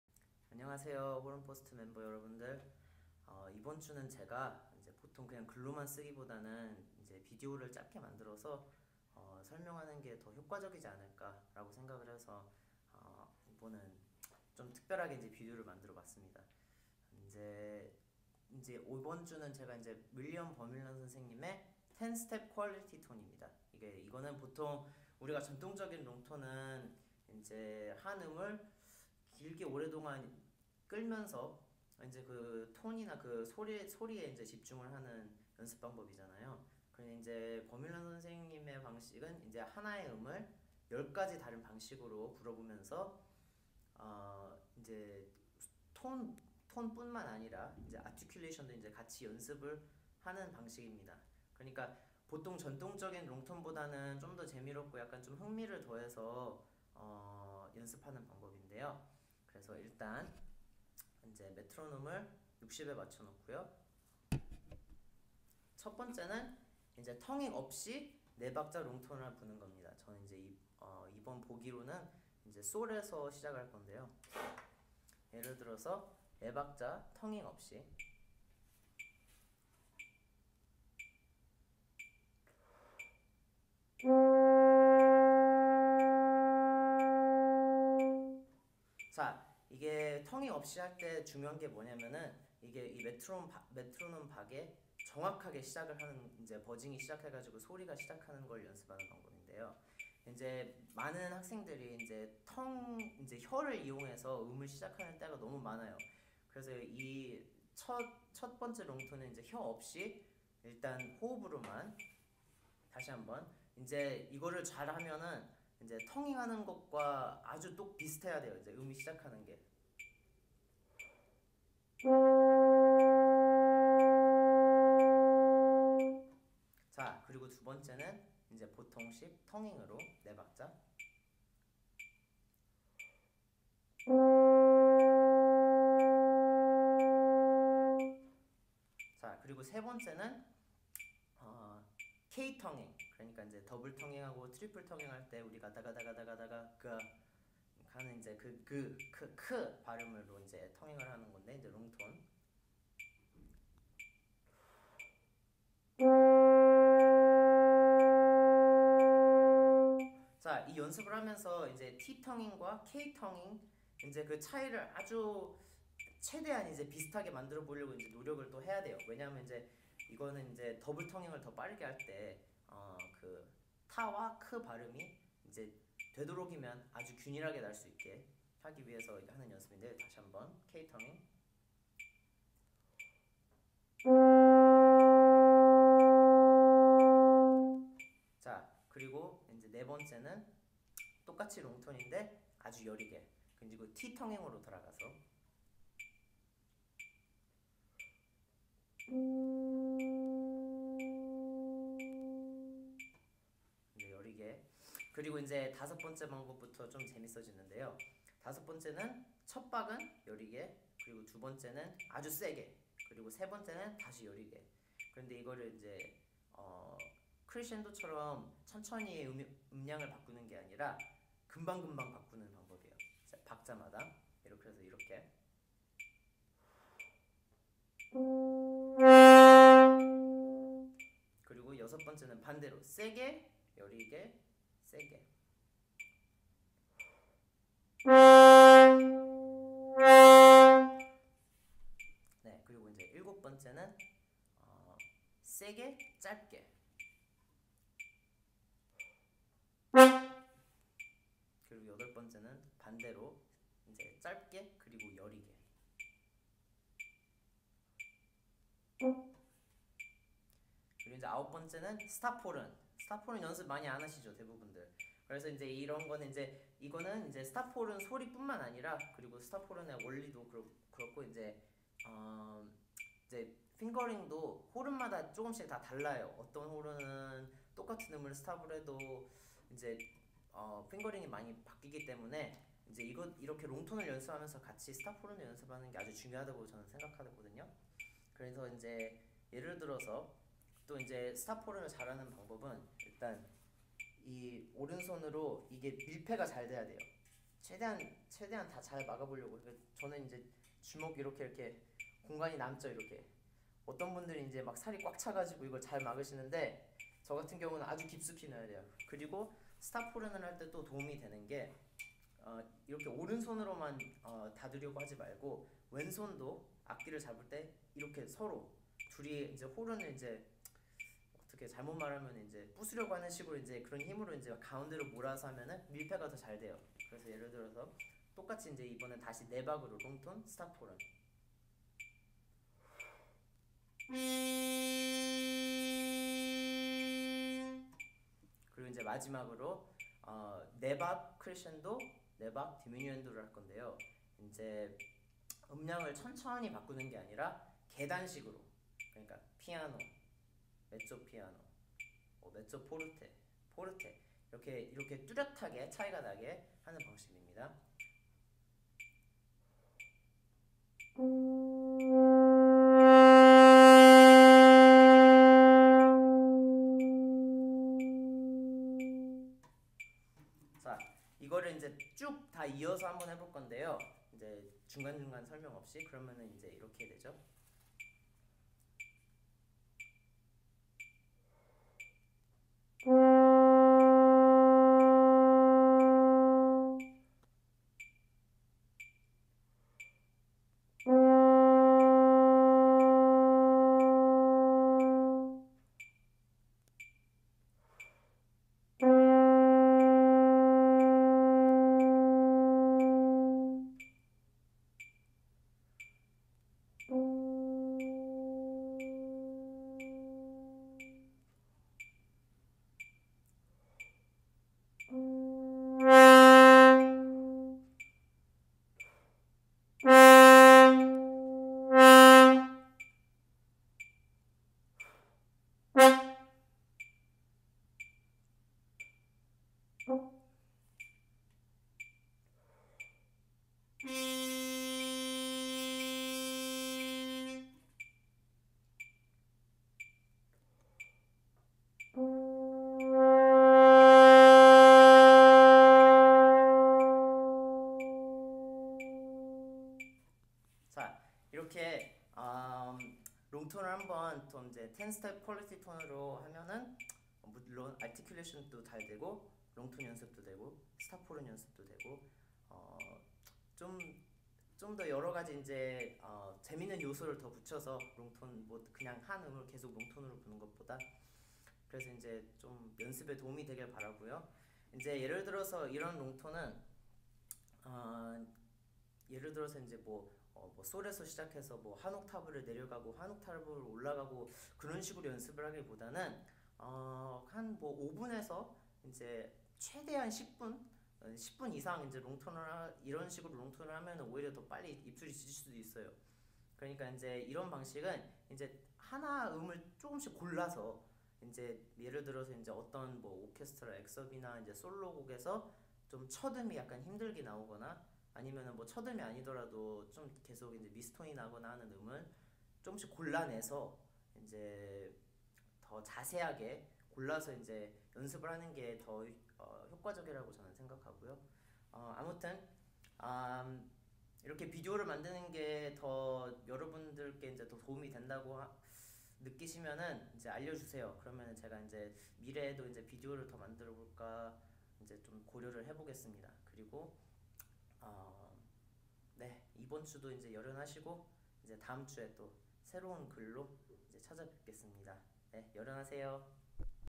호른팁 열넷째 주: 윌리엄 버뮬런의 10-스텝 퀼리티 톤
전통적인 롱톤 대신에 한 음을 10가지 종류로 연습하는 방법입니다.

1. 텅잉 없이 롱톤 4박
2. 보통 텅잉으로 시작하는 롱톤 4박
3. 더블 트리플 텅잉에 쓰는 K텅으로 시작하는 롱톤 4박
4. 보통 텅이으로 여리게 롱톤 4박
5. 1박 여리게 - 1박 세게 - 1 박 여리게 (총3박)
6. 1박 세게 - 1박 여리게 - 1박 세게 (총3박)
7. 1박 세게, 스타카토
8. 1박 여리게, 스타카토
9. 스탑 호른 롱톤 4박
10. 4박 크레센도 - 4박 디메뉴엔도- 매박 계단식 음량을 높이고 낮추세요